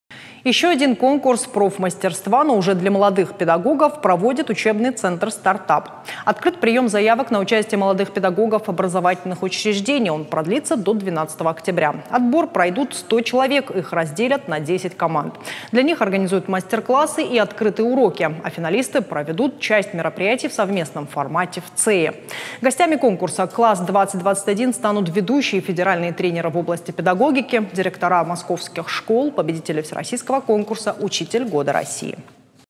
В Северной Осетии стартовал приём заявок на конкурс профмастерства для молодых педагогов
Ещё один конкурс профмастерства, но уже для молодых педагогов проводит учебный центр «Стартап». Открыт приём заявок на участие молодых педагогов образовательных учреждений. Он продлится до 12 октября. Отбор пройдут 100 человек. Их разделят на 10 команд.

Для них организуют мастер-классы и открытые уроки. А финалисты проведут часть мероприятий в совместном формате в Цее. Гостями конкурса «Класс-2021» станут ведущие федеральные тренеры в области педагогики, директора московских школ, победители Всероссийского конкурса «Учитель года России».
Мы рядом в:
ok.ru/irystontv